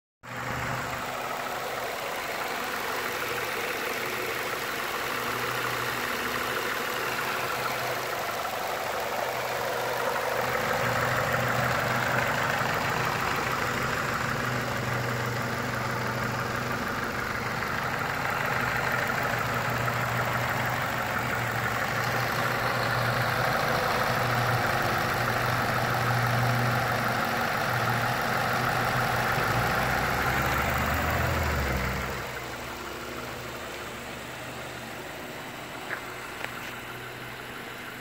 2009 Ford Fusion 2.3l engine run test
Testing the engine on this 2009 Ford Fusion before it is stripped for parts.